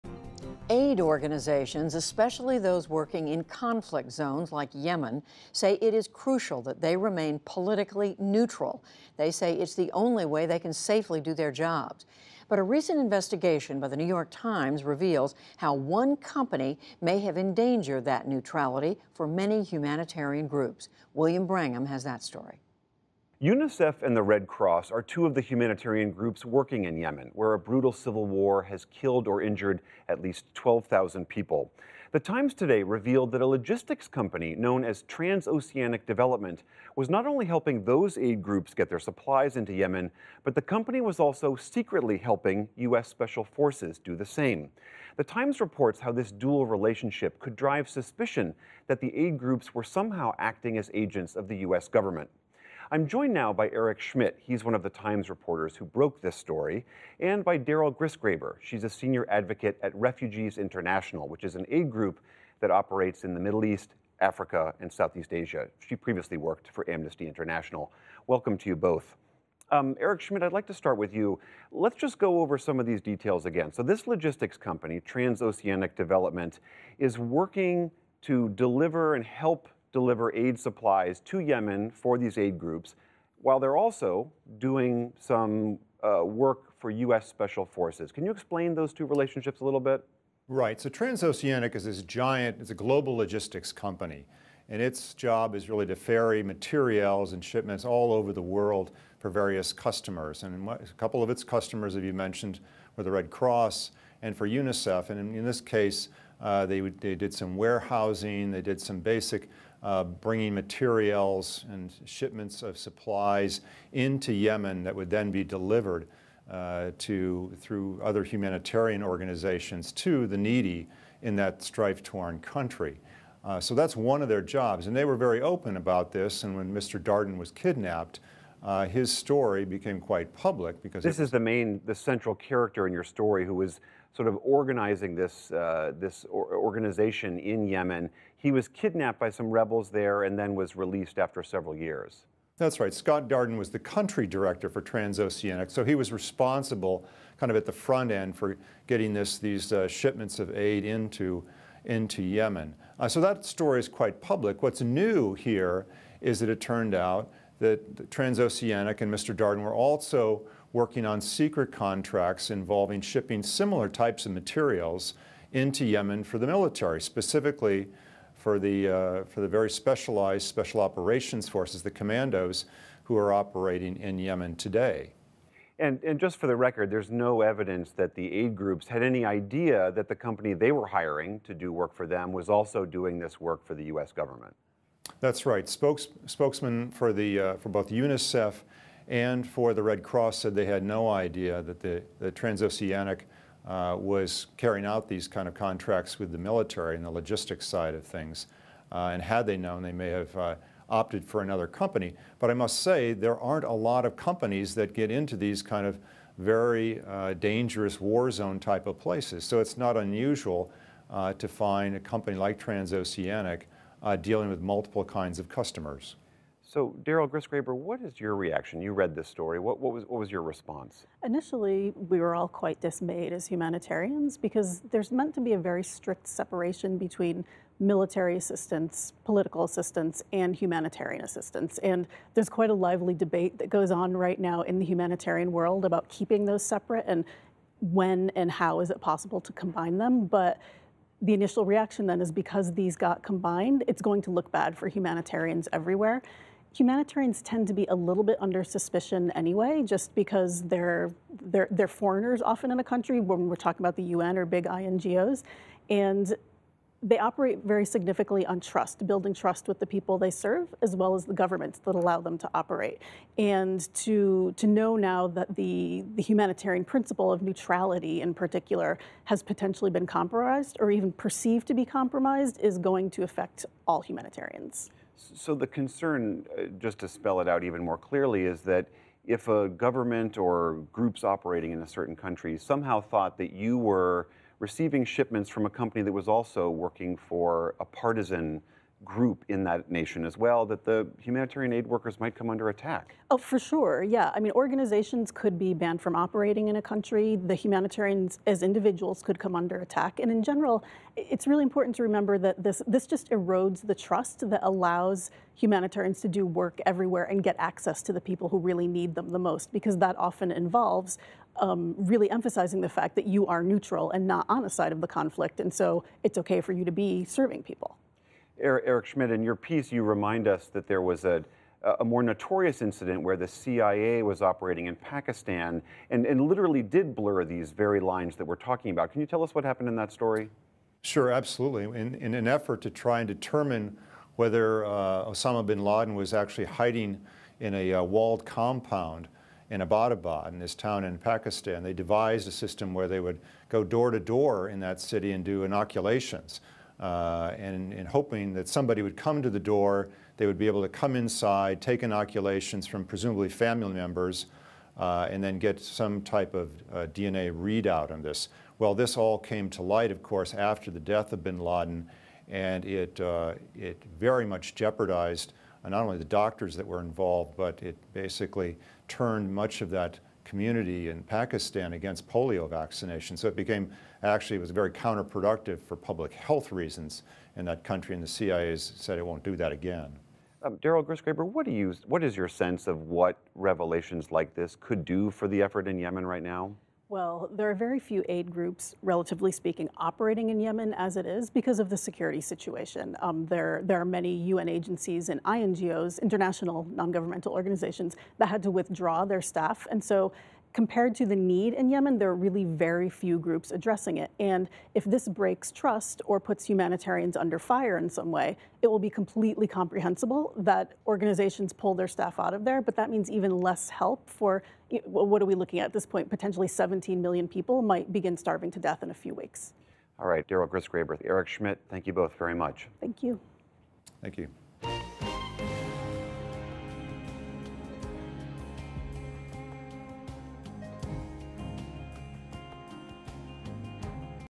Will one company’s mixed mission in Yemen sow suspicion of aid groups?
For aid organizations, especially those in conflict zones, remaining politically neutral is crucial for trust. A New York Times investigation found that the conduct of a logistics company could drive suspicion that aid groups in Yemen were somehow acting as agents of the U.S. government. William Brangham speaks with The New York Times' Eric Schmitt and Daryl Grisgraber of Refugees International.